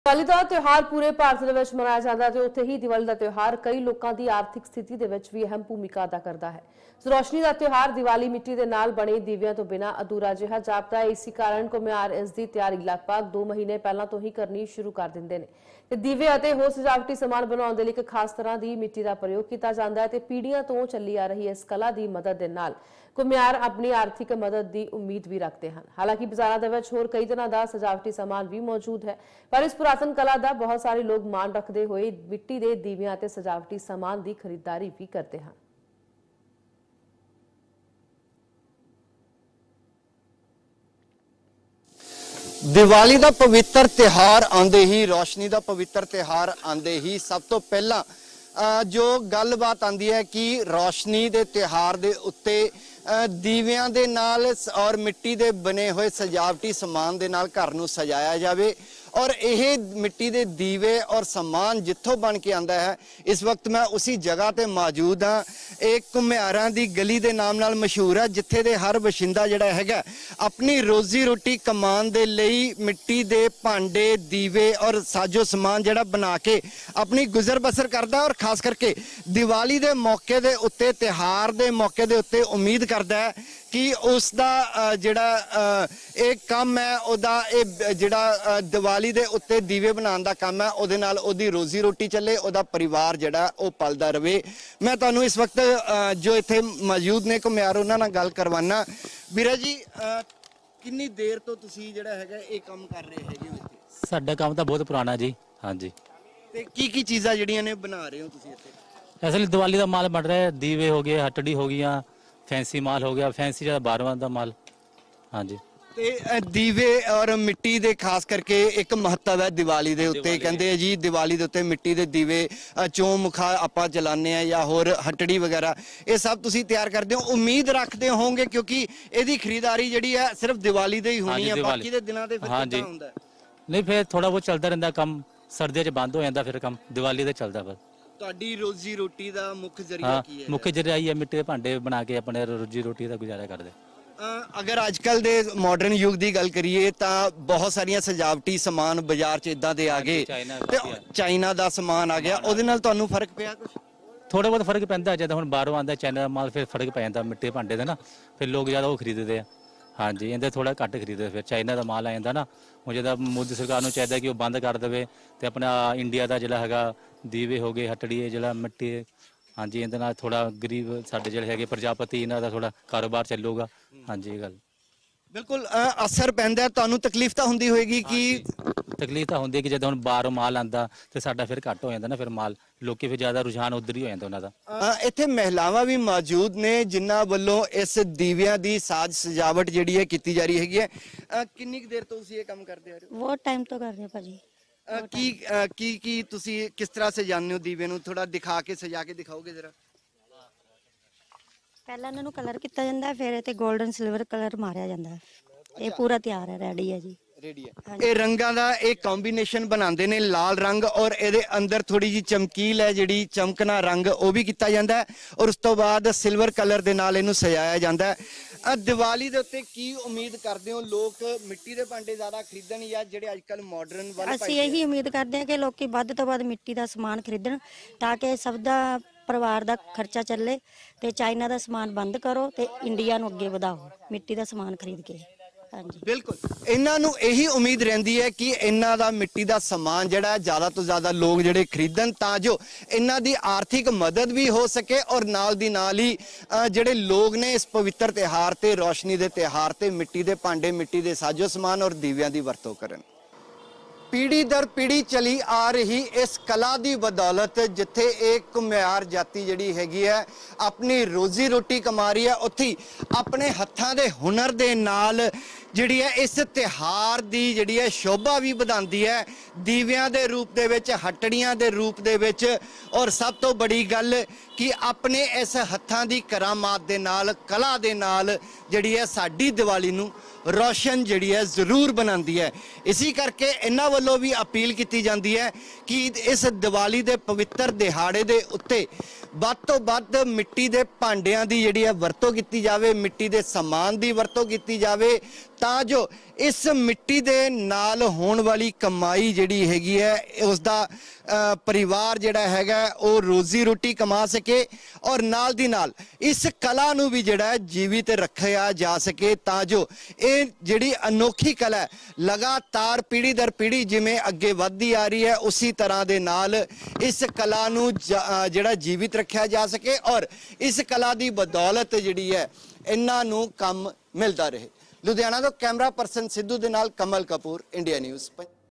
Diwali | Festival | Punjab | Ludhiana ਮਿੱਟੀ ਨਾਲ ਬਣੇ ਦੀਵਿਆਂ ਤੋਂ ਬਿਨਾਂ ਅਧੂਰਾ ਤਿਉਹਾਰ INDIA NEWS PUNJAB
ਦਿਵਾਲੀ ਦਾ ਤਿਉਹਾਰ ਪੂਰੇ ਭਾਰਤ ਵਿੱਚ ਮਨਾਇਆ ਜਾਂਦਾ ਹੈ। ਉੱਥੇ ਹੀ ਦਿਵਾਲੀ ਦਾ ਤਿਉਹਾਰ ਕਈ ਲੋਕਾਂ ਦੀ ਆਰਥਿਕ ਸਥਿਤੀ ਵਿੱਚ ਵੀ ਅਹਿਮ ਭੂਮਿਕਾ ਅਦਾ ਕਰਦਾ ਹੈ। ਰੋਸ਼ਨੀ ਦਾ ਤਿਉਹਾਰ ਦਿਵਾਲੀ ਮਿੱਟੀ ਨਾਲ ਬਣੇ ਦੀਵਿਆਂ ਤੋਂ ਬਿਨਾਂ ਅਧੂਰਾ ਜਿਹਾ ਜਾਪਦਾ ਹੈ।

The festival of Diwali is celebrated all over India. At the same time, the festival of Diwali also plays an important role in the economic condition of many people. Diwali festival of lights seems incomplete without the clay lamps.